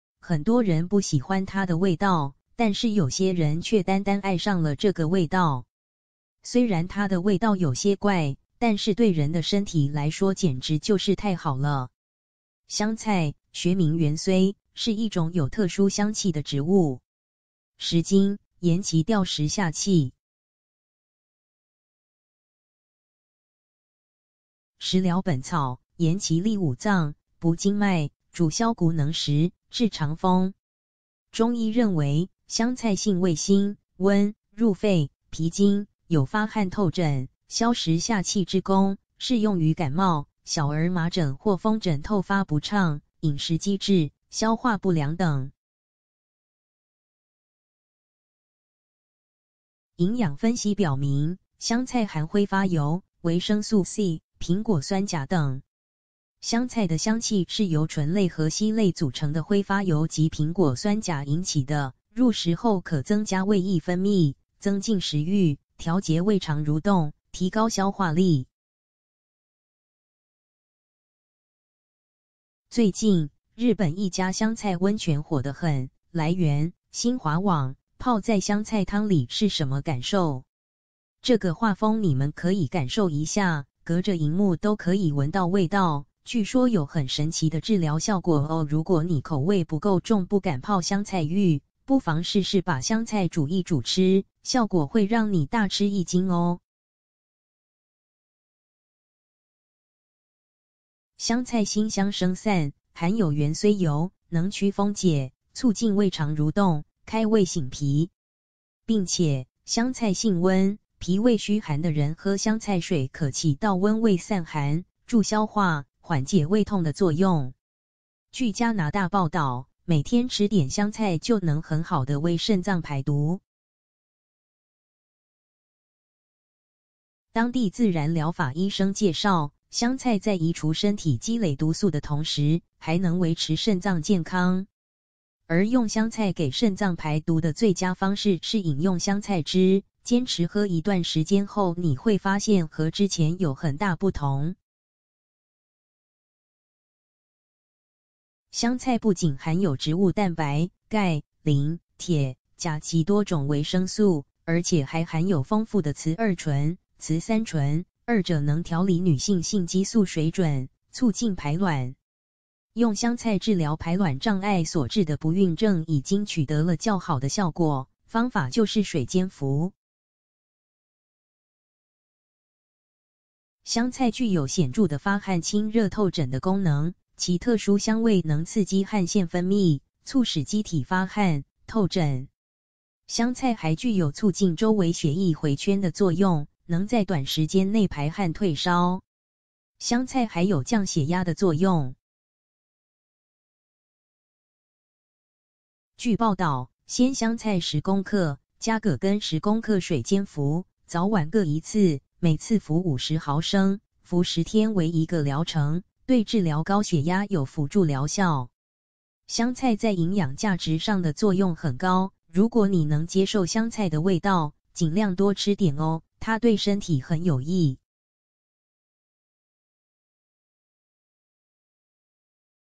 香菜用水煎後食用，能改善你身體上的5種頑固疾病
很多人不喜歡它的味道，但是有些人卻單單愛上了這個味道。  雖然它的味道有些怪，但是對人的身體來說簡直就是太好了。  香菜，學名芫荽，是一種有特殊香氣的植物。   《食經》言其「調食下氣」。     《食療本草》言其「利五臟，補筋脈，主消穀能食，治腸風」。中醫認為，香菜性味辛、溫，入肺、脾經，有發汗透疹，消食下氣之功，適用于感冒、小兒麻疹或風疹透發不暢、飲食積滯、消化不良等。營養分析表明，香菜含揮發油、維生素C、蘋果酸鉀等。   香菜的香氣是由醇類和烯類組成的揮發油及蘋果酸鉀引起的，入食後可增加胃液分泌，增進食欲，調節胃腸蠕動，提高消化力。  最近，日本一家「香菜溫泉」火的很（來源：新華網），泡在香菜湯裡是什麼感受？這個畫風你們可以感受一下，隔著螢幕都可以聞到味道，據說有很神奇的治療效果哦~~如果你口味不夠重不敢泡香菜浴，不妨試試把香菜煮一煮吃，效果會讓你大吃一驚哦。  香菜辛香升散，含有芫荽油，能驅風解、促進胃腸蠕動、開胃醒脾。   並且，香菜性溫，脾胃虛寒的人喝香菜水可起到溫胃散寒、助消化、緩解胃痛的作用。     據加拿大報導，每天吃點香菜就能很好地為腎臟排毒。   當地自然療法醫生介紹，香菜在移除身體積累毒素的同時，還能維持腎臟健康。   而用香菜給腎臟排毒的最佳方式是飲用香菜汁，堅持喝一段時間後你會發現和之前有很大不同。  香菜不僅含有植物蛋白、鈣、磷、鐵、鉀及多種維生素，而且還含有豐富的雌二醇、雌三醇，二者能調理女性性激素水準，促進排卵。   用香菜治療排卵障礙所致的不孕症已經取得了較好的效果，方法就是水煎服。    香菜具有顯著的發汗清熱透疹的功能，其特殊香味能刺激汗腺分泌，促使機體發汗，透疹。   香菜還具有促進周圍血液迴圈的作用，能在短時間內排汗退燒。     香菜還有降血壓的作用。據報導，鮮香菜10g，加葛根10g水煎服，早晚各1次，每次服50ml，服10天為1個療程，對治療高血壓有輔助療效。    香菜在營養價值上的作用很高，如果你能接受香菜的味道，儘量多吃點哦，它對身體很有益。